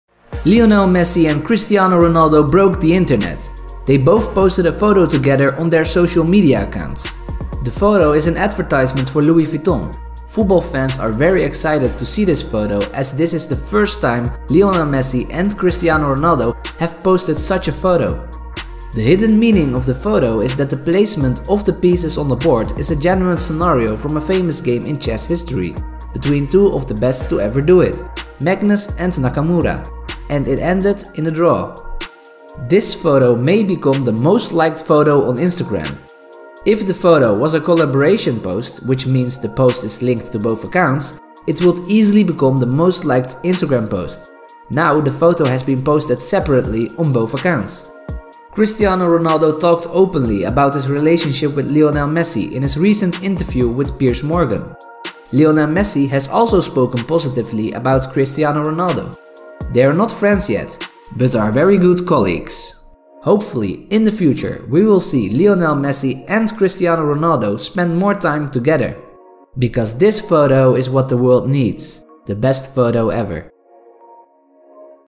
Lionel Messi & Cristiano Ronaldo BROKE The Internet! (WORLD RECORD)
Lionel Messi and Cristiano Ronaldo broke the internet. They both posted a photo together on their social media accounts @leomessi @cristiano (Instagram, Facebook and Twitter). Football fans are very excited to see this photo.

In this video we will explain why the photo was made (just before the World Cup 2022 in Qatar), the hidden meaning behind the photo of Lionel Messi and Cristiano Ronaldo.

The photo chess game photo of Lionel Messi and Cristiano Ronaldo may become the most liked photo on Instagram. Lionel Messi and Cristiano Ronaldo are becoming great friends!

Follow KYSTAR!